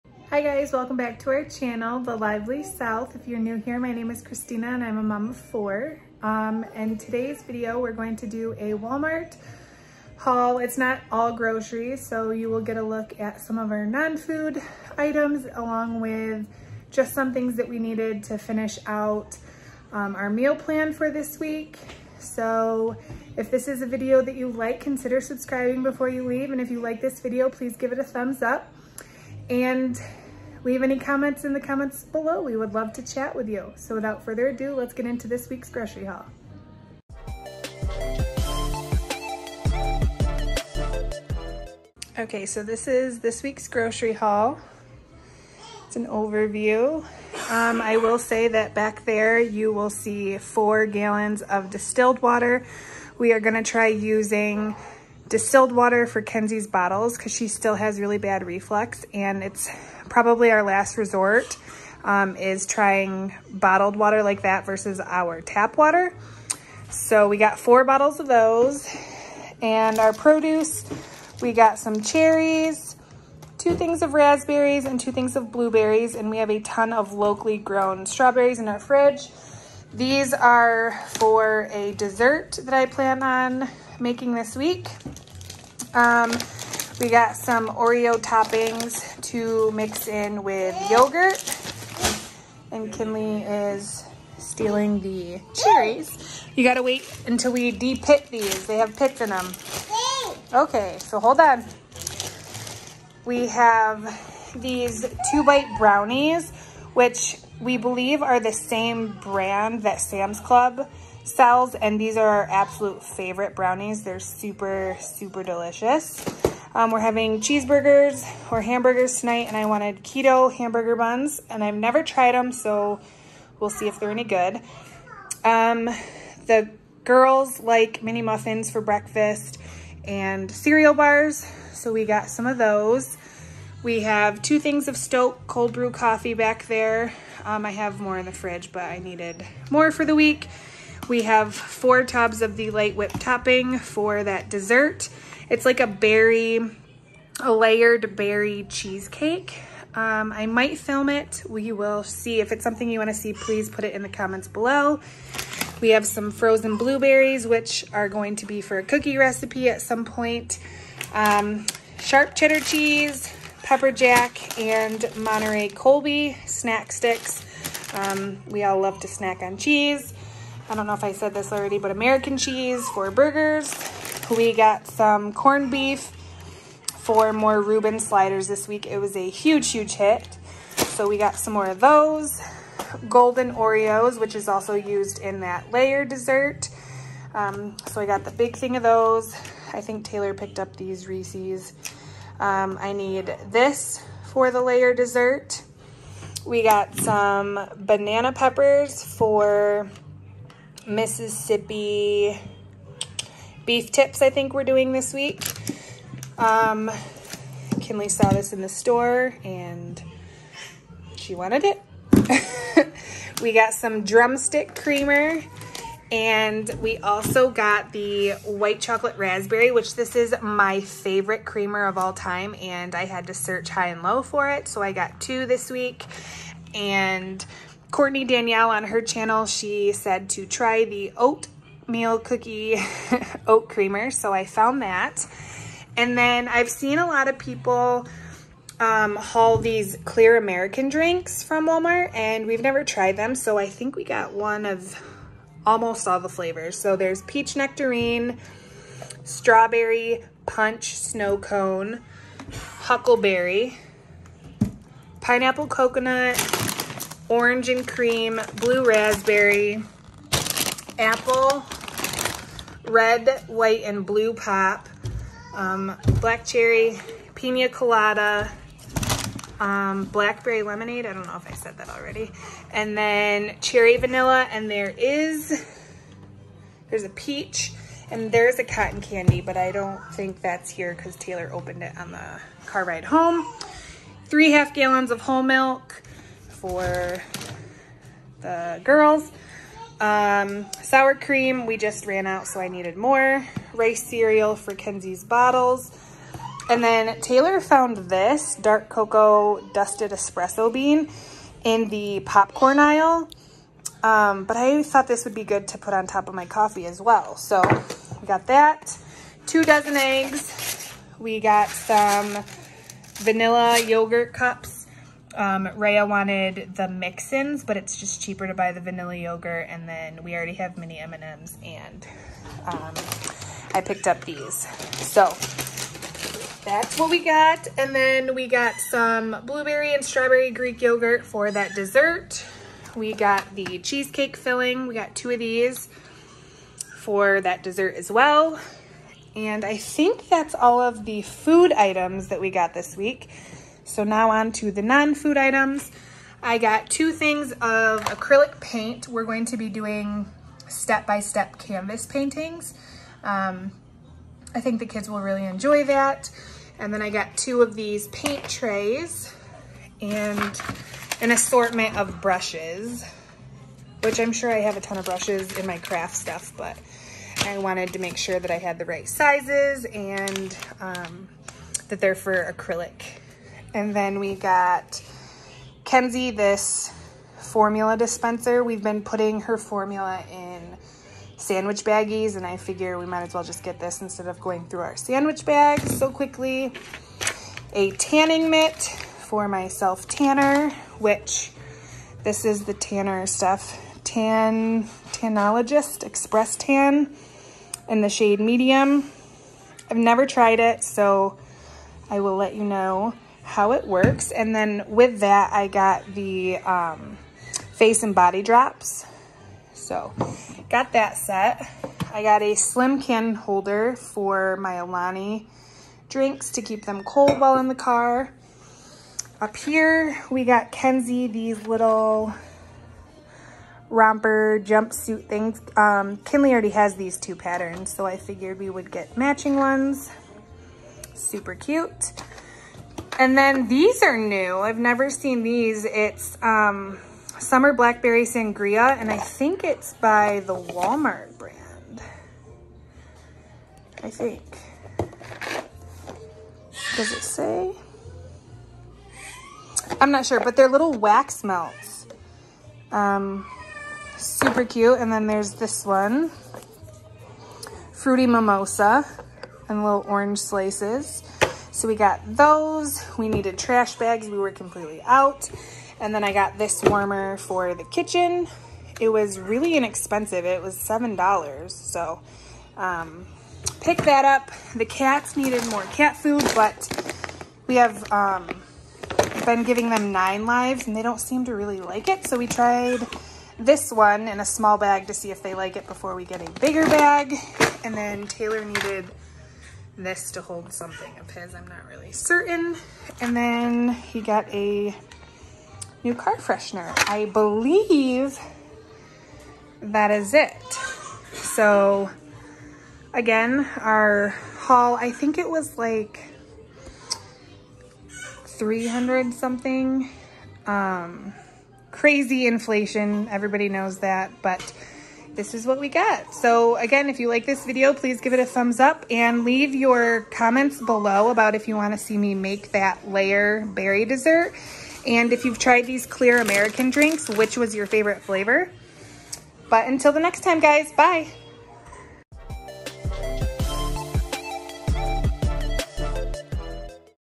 Walmart Grocery Haul | NON FOOD ITEMS | MEAL PLAN | MOM OF 4
Hi family, on todays video you’ll see our grocery haul as well as some non food items hauled! It’s crazy how expensive groceries have gotten! Hope you’re all doing well and are healthy! I hope you enjoy todays haul!

TheLivelySouth